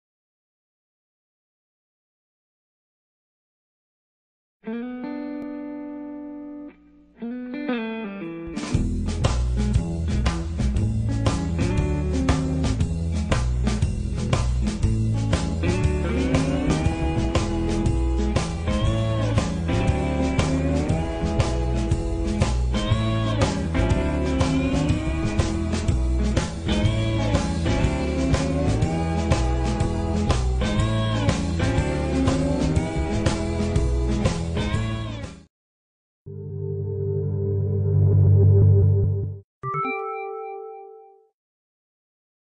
Ever seen a cat bring a stick?!
A cat bringing a stick! LOL!